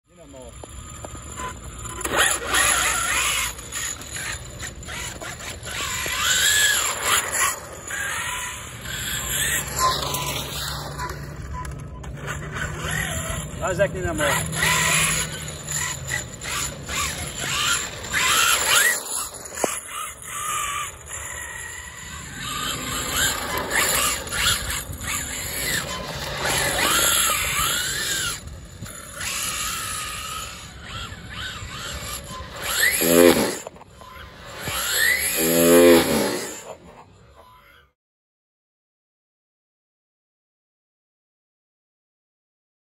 Bigscale RC in the harbour
Elcon Models rc 1/5 bigscale brushless
Max 5 esc
HW 800kv motor

Tires cant handle full throttle…